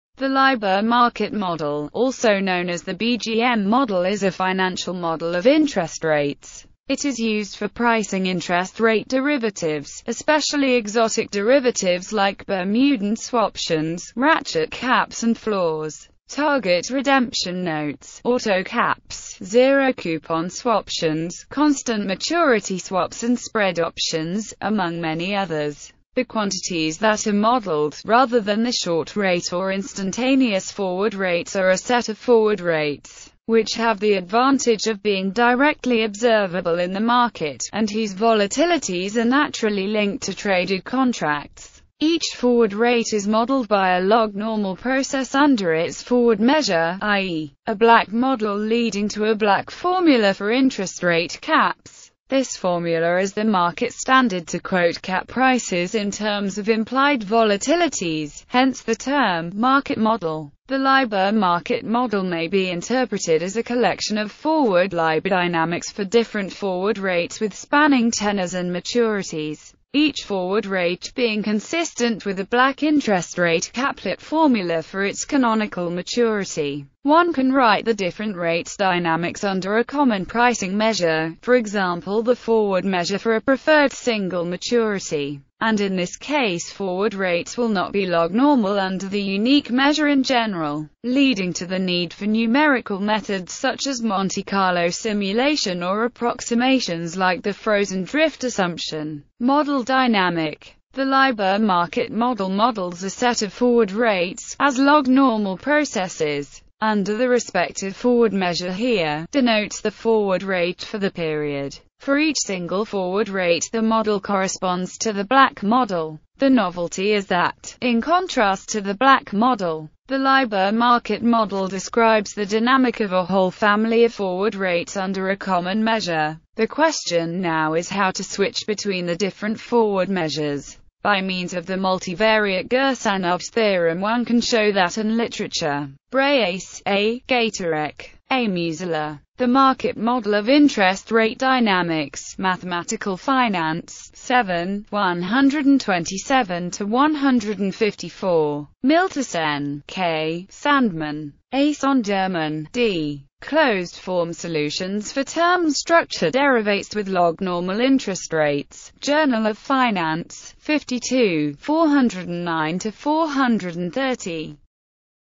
LIBOR market model
The LIBOR market model, also known as the BGM Model (Brace Gatarek Musiela Model, in reference to the names of some of the inventors) is a financial model of interest rates.It is used for pricing interest rate derivatives, especially exotic derivatives like Bermudan swaptions, ratchet caps and floors, target redemption notes, autocaps, zero coupon swaptions, constant maturity swaps and spread options, among many others.